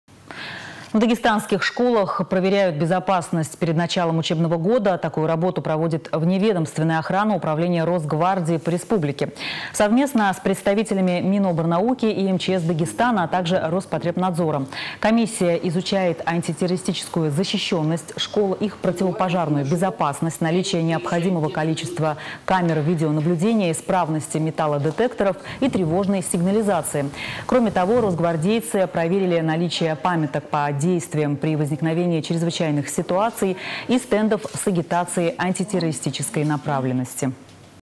В Дагестанских школах проверяют безопасность перед началом учебного года
Такую работу проводит вневедомственная охрана Управления Росгвардии по республике. Совместно с представителями Минобрнауки и МЧС Дагестана, а также Роспотребнадзора.